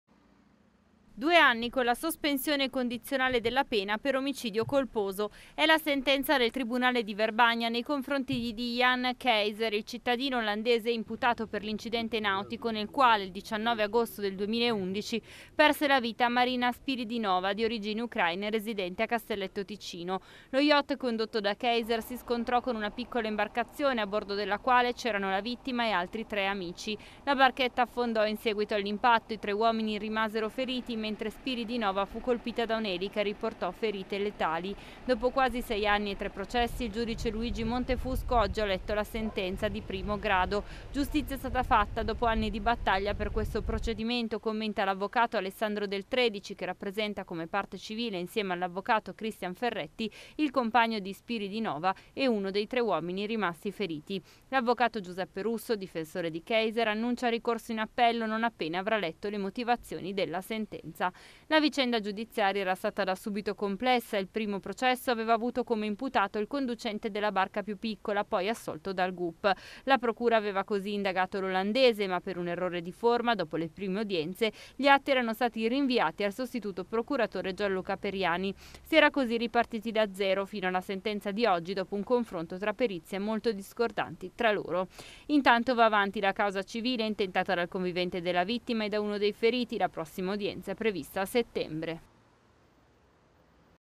Naufragio Belgirate, due anni al conducente dello yacht
8 giugno 2017 - Incidente nautico di Belgirate: condanna a due anni con la sospensione condizionale per l'olandese al timone dello yacht